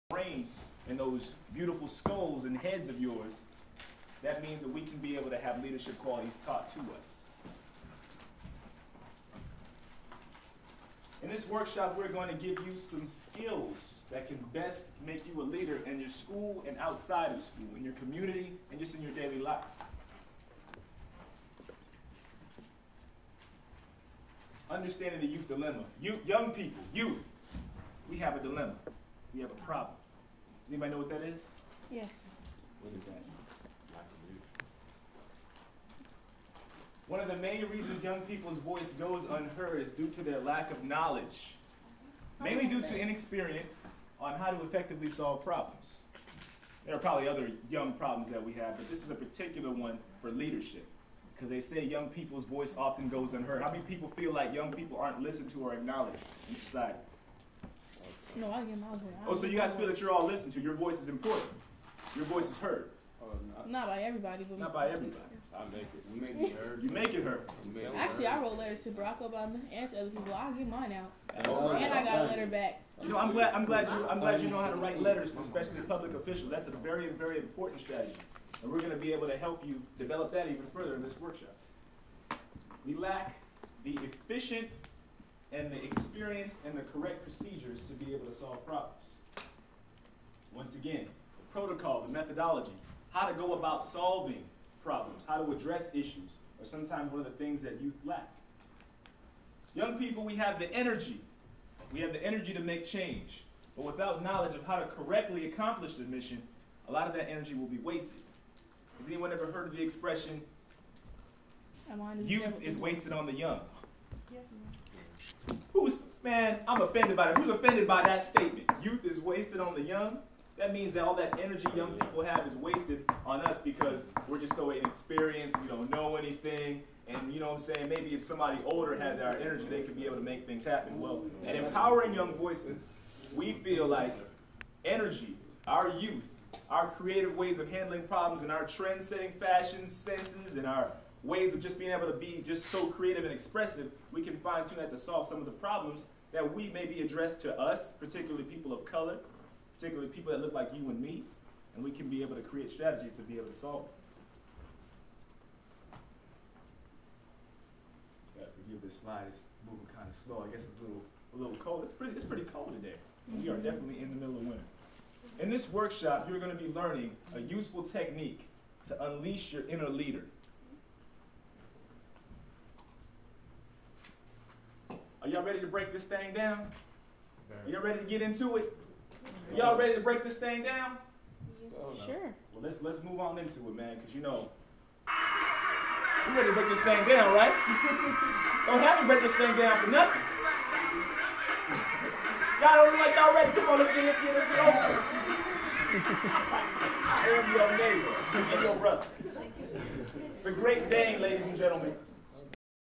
EYV Great D.A.N.E Leadership Workshop pt. 2.AVI
Empowering Young Voices, Inc. (EYV) is a non-profit organization based in Stockton, CA. This organization was created by young college students and community youth who wanted to change the scope of education for young people.  Particularly young people from disadvantaged backgrounds.  The organization teaches life-skills and leadership training.  The type of training and skills you may not always receive in the classroom.  This is a sample workshop, prepared and facilitated by the organization's Founder & Executive Director; Brandie Spencer.  In this workshop, the attendees are learning a leadership technique called "The Great D.A.N.E" This workshop is being held at the University of the Pacific (Stockton, CA) where Brandie Spencer attends.